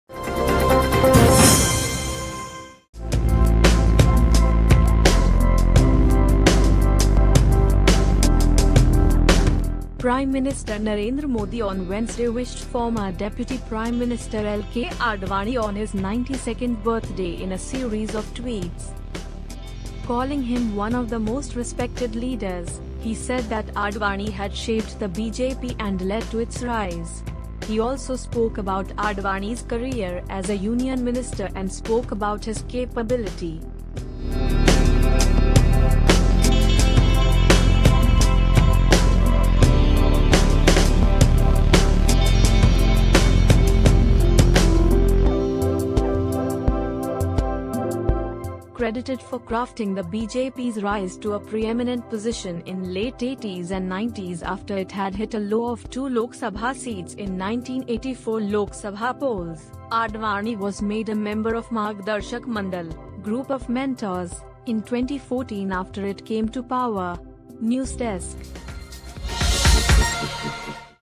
PM Modi wishes LK Advani on his 92nd birthday
PM Narendra Modi on Wednesday wished former Deputy Prime Minister LK Advani on his 92nd birthday in a series of tweets. Calling him one of the most respected leaders, he said that Advani had shaped the BJP and led to its rise. He also spoke about Advani's career as a Union Minister and spoke about his capability. Credited for crafting the BJP's rise to a pre-eminent position in late 80s and 90s after it had hit a low of two Lok Sabha seats in 1984 Lok Sabha polls, Advani was made a member of 'margdarshak mandal' (group of mentors) in 2014 after it came to power.